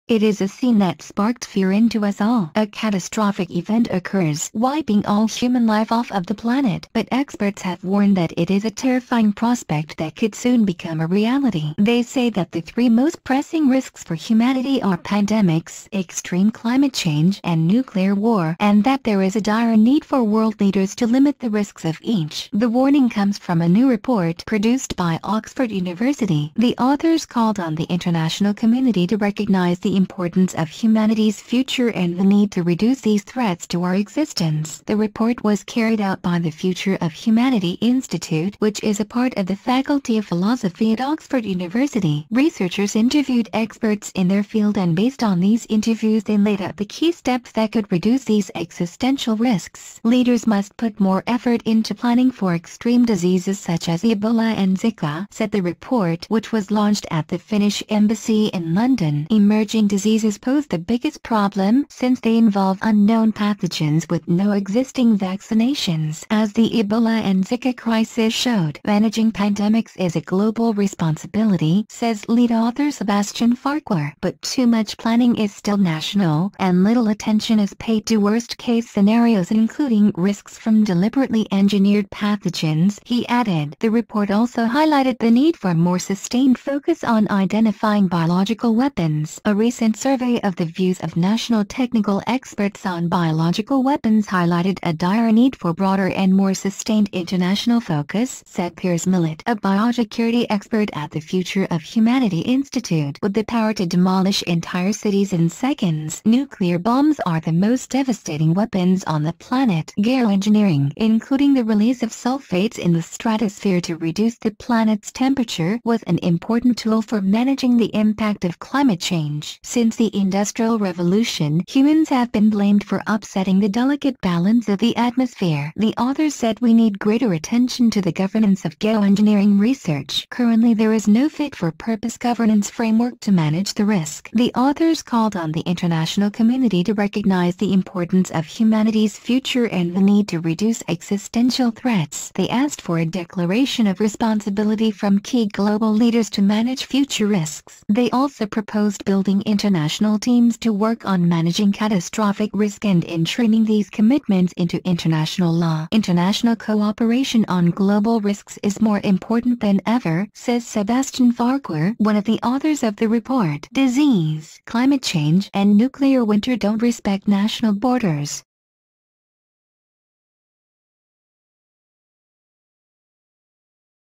The three theories of the APOCALYPSE
It is a scene that sparks fear into us all - a catastrophic event occurs, wiping all human life off of the planet.
But experts have warned that it is a terrifying prospect that could soon become a reality.